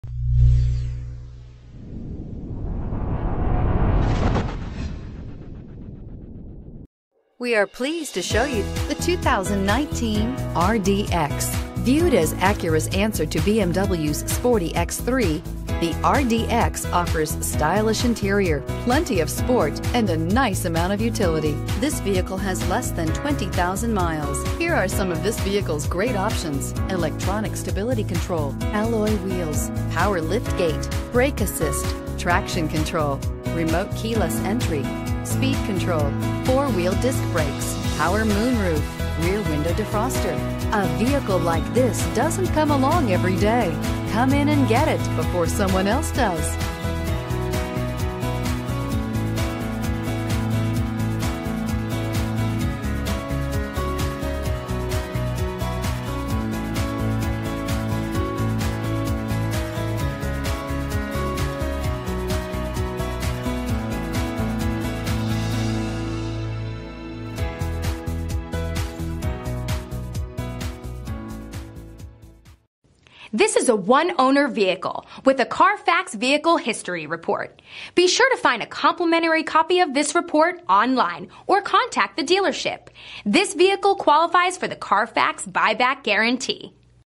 2019 Acura RDX MONTEREY PARK ROSEMEAD EL MONTE MONTEBELLO LOS ANGELES A26631
2019 Acura RDX AWD

Alhambra Acura
1700 W. Main St.

New Price! White 2019 Acura RDX SH-AWD AWD 10-Speed Automatic 2.0L 16V DOHC CPO/Certified, Moonroof/Sunroof, Leather, CarFax One Owner, Clean Carfax, Bluetooth, Backup Camera. Recent Arrival!